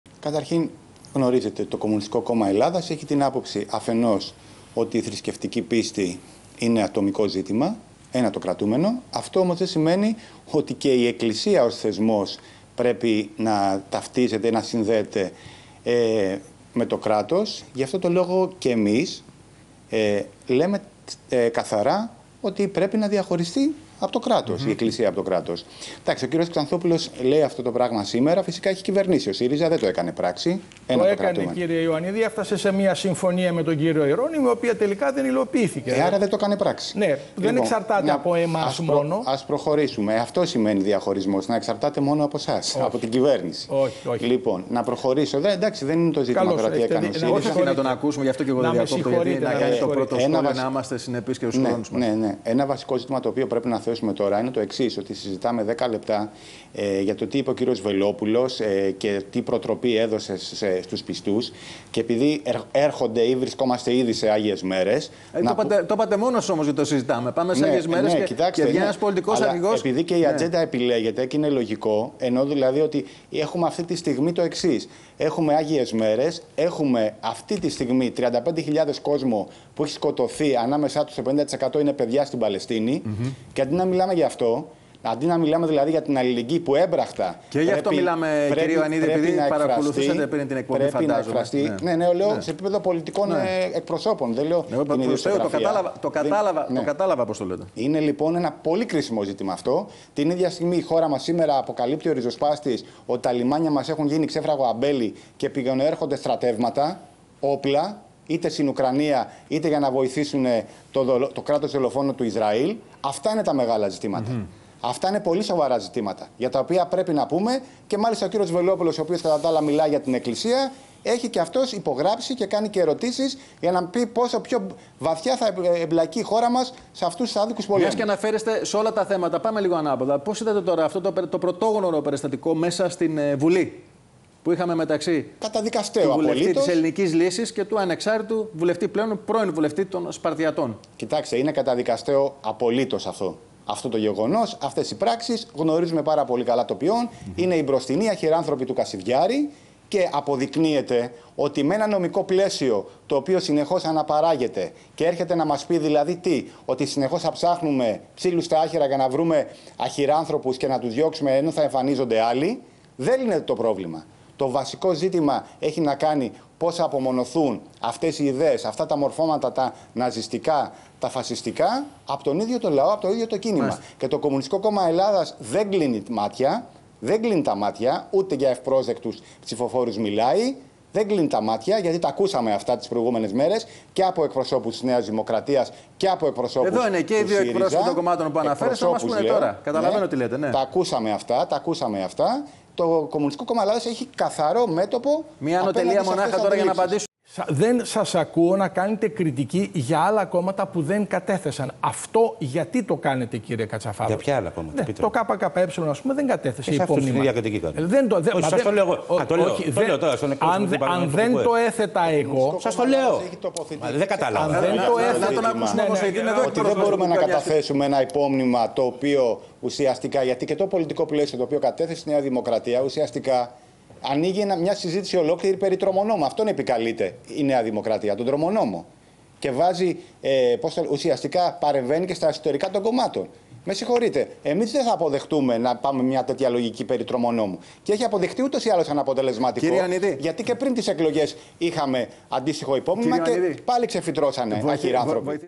Κ. Ιωαννίδης: Το βασικό είναι να απομονωθούν ναζιστικά φασιστικά μορφώματα από τον ίδιο τον λαό
Σε τηλεοπτική εκπομπή της ΕΡΤ News συμμετείχε το πρωί της Παρασκευής, ο Κυριάκος Ιωαννίδης, μέλος της ΚΕ του ΚΚΕ.

Κληθείς να σχολιάσει τις δηλώσεις Βελόπουλου προς τους βουλευτές που ψήφισαν τον γάμο των ομόφυλων ζευγαριών «να μην πάνε στην εκκλησία το Πάσχα», ο Κ. Ιωαννίδης υπενθύμισε ότι το ΚΚΕ έχει την άποψή ότι η θρησκευτική πίστη είναι ατομικό ζήτημα και ότι πρέπει να διαχωριστεί από το κράτος η εκκλησία από το κράτος.

Ωστόσο, κατέκρινε το γεγονός ότι την ώρα που έχουμε 35.000 νεκρούς, ανάμεσά τους και πολλά παιδιά στην Παλαιστίνη, τη στιγμή που όπως αποκαλύπτει ο Ριζοσπάστης, τα λιμάνια μας έχουν γίνει ξέφραγο αμπέλι και πηγαινοέρχονται στρατεύματα, είτε στην Ουκρανία είτε για να βοηθήσουν το κράτος τηλέφωνο του Ισραήλ, ορισμένοι θέλουν η συζήτηση να περιστρέφεται γύρω από δηλώσεις όπως αυτές του Κ. Βελόπουλου.

Για το περιστατικό μέσα στην Βουλή μεταξύ του βουλευτή της Ελληνικής Λύσης και του πρώην βουλευτή των Σπαρτιατών, ο Κ. Ιωαννίδης τόνισε ότι είναι καταδικαστέο και πρόσθεσε πως αποδεικνύεται ότι τέτοια φαινόμενα δεν αντιμετωπίζονται αποτελεσματικά μέσα από το «νομικό πλαίσιο» αλλά το βασικό ζήτημα έχει να κάνει με το πώς θα απομονωθούν αυτές οι ιδέες, αυτά τα ναζιστικά φασιστικά μορφώματα, από τον ίδιο το λαό από το ίδιο το κίνημα.